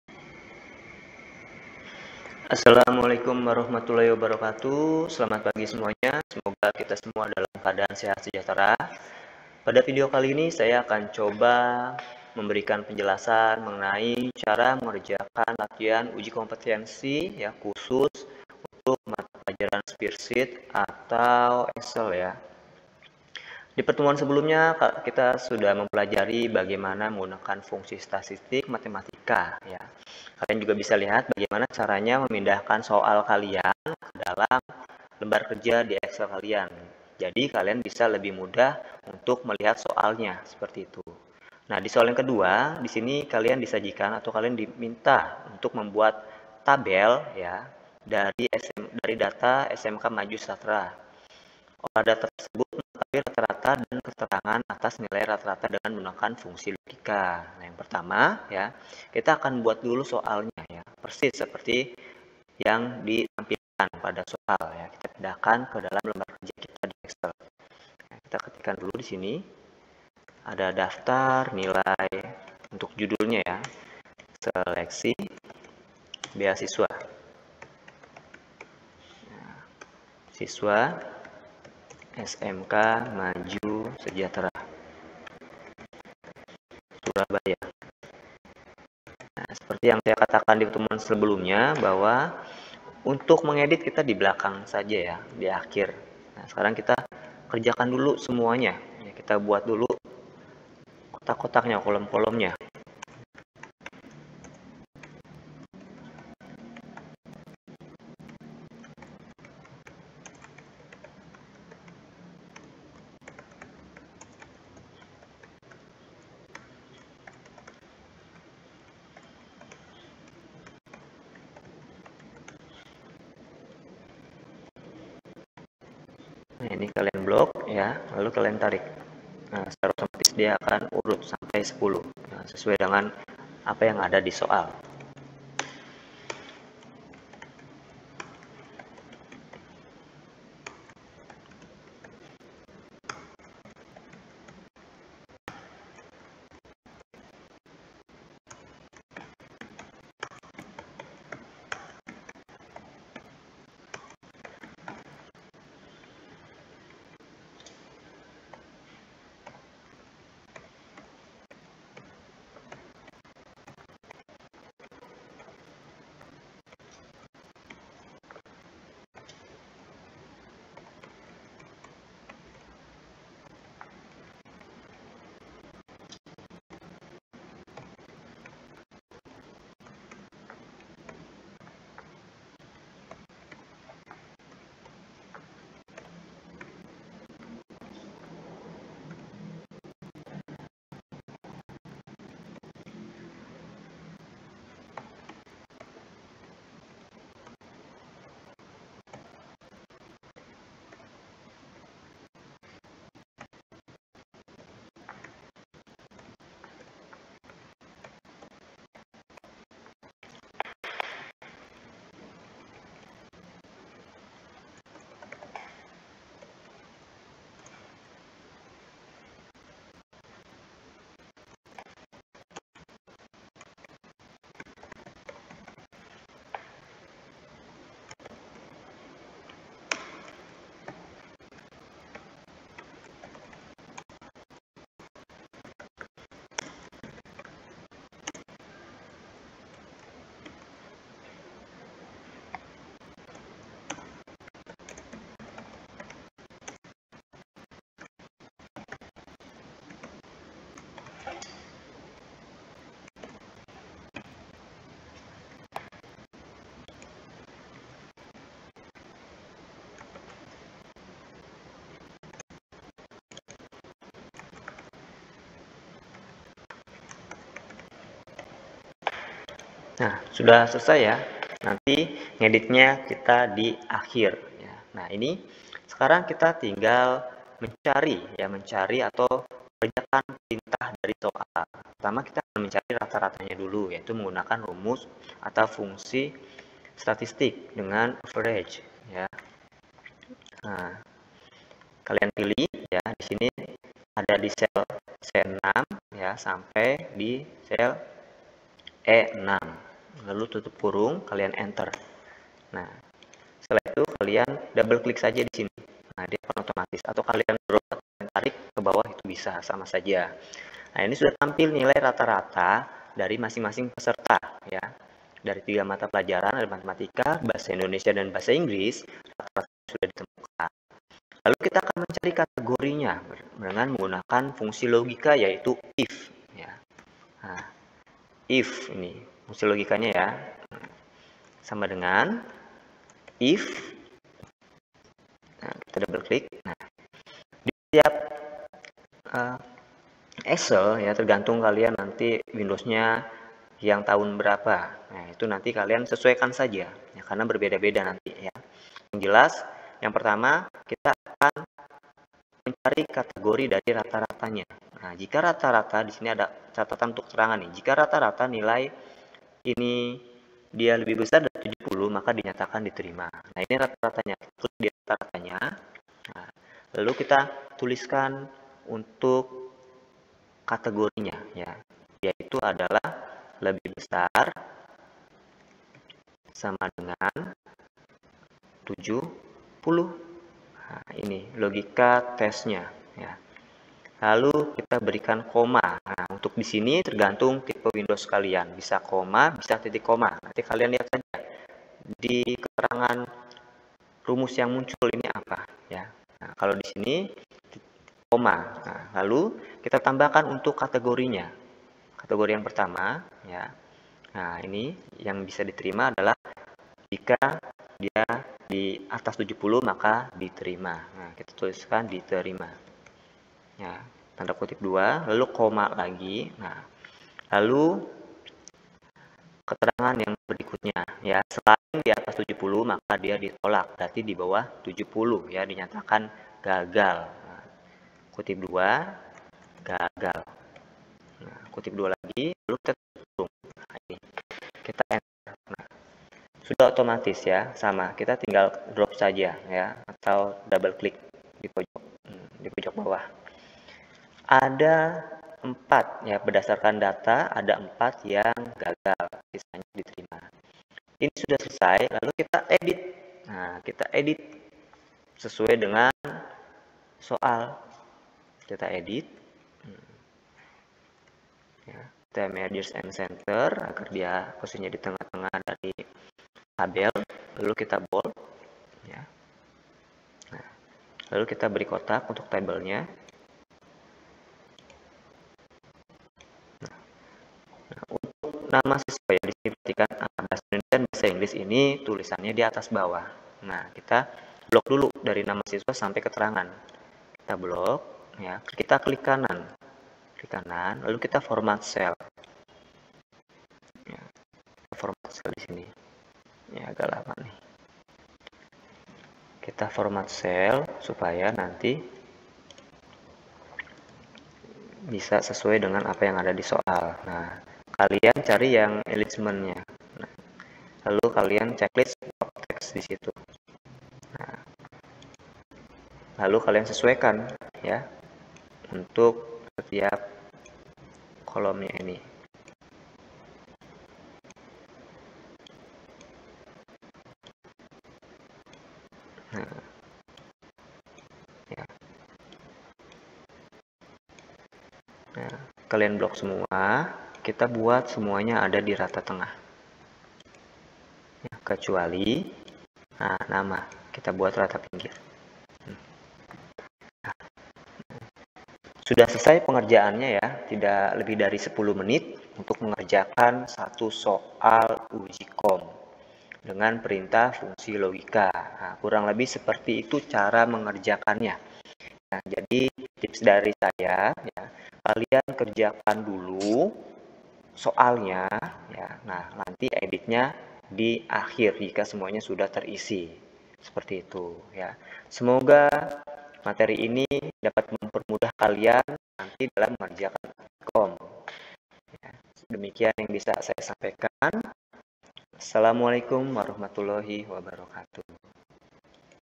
Latihan Soal Uji Kompetensi Mapel SpreadSheet Bag 2
Assalamualaikum,
Video ini berisikan lanjutan dari penjelasan soal Ujikompetensi untuk mata pelajaran Spreadsheet mengenai fungsi Logika yaitu IF.